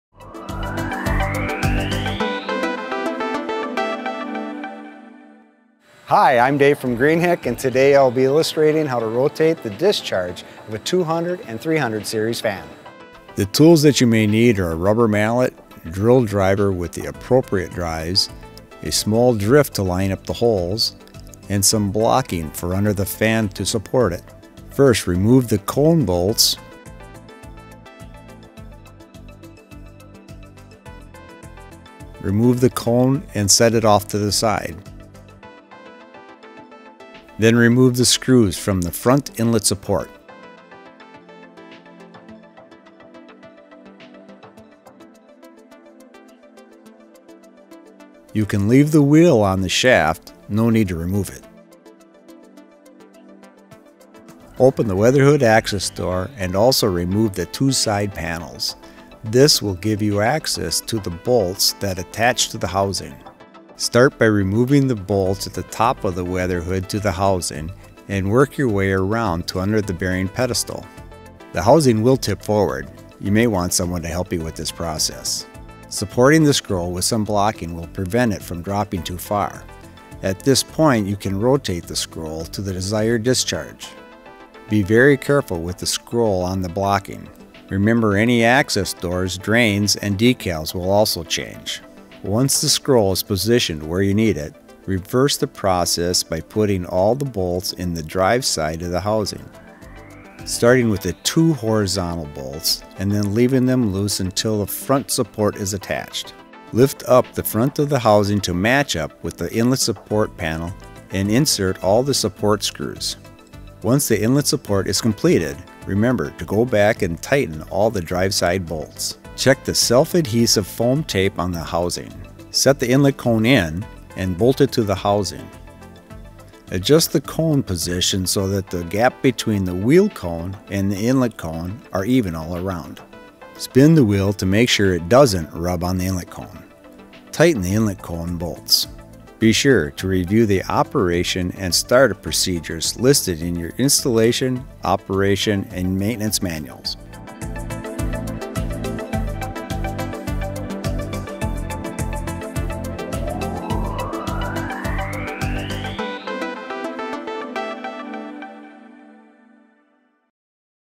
Greenheck - How To Rotate the Discharge of a 200 & 300 Series Fan
The tools that you may need are a rubber mallet, drill driver with the appropriate drives, a small drift to line up the holes and some blocking for under the fan to support it.

First remove all the cone bolts, removed the cone and set off to the side.

Then remove all the screws from the front inlet support.

You can leave the wheel on the shaft, no need to remove it.

Open the weatherhood access door and remove the two side panels. This will give you access to the bolts from the base to the housing.

Start by removing the bolts at the top of the weather hood to the housing and work your way down around to under the bearing pedestal.

The housing will tip forward. You may want someone to help with this process.

Supporting the scroll with some blocking will prevent it from dropping to far. At this point you can rotate the scroll to the desired discharge. (Be very careful with the scroll on the blocking.)

(You CANNOT change direction!!)

Remember, any access doors, drains and decals will also change.

Once the scroll is positioned where you need it, and then reverse the process by putting all of the bolts in the back of the housing.  Staring with the two horizontal bolts and leaving them loose till the front support is attached.

Lift up the front of the housing to match up with the inlet support panel and insert all the support screws.
Once the inlet support is completed, remember to go back and tighten all the drive side bolts.

Check the self-adhesive foam tape on the housing.

Set the inlet cone in and bolt it to the housing. Adjust the cone position so that the gap between the wheel cone and the inlet cone is even all around.

Spin the wheel to make sure it does not rub on the inlet cone. Tighten inlet cone bolts.

Be sure to review the operation and start-up procedures listed in your installation, operation and maintenance manual.